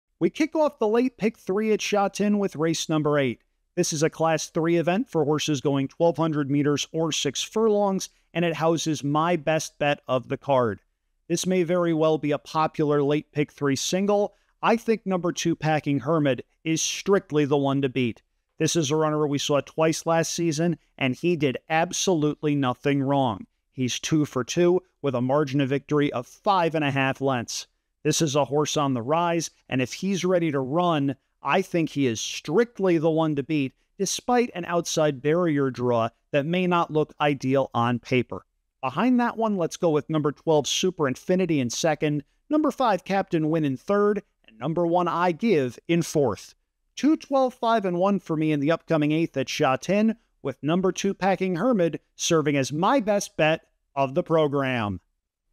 Andrew Champagne Quickfire Preview - Best Bet
Andrew Champagne's Best Bet for Sha Tin 10/20/24, Race 8, Hong Kong Racing

✓ DRF Bets: Get started playing the races with a $200 Deposit Match & Free DRF Past Performances.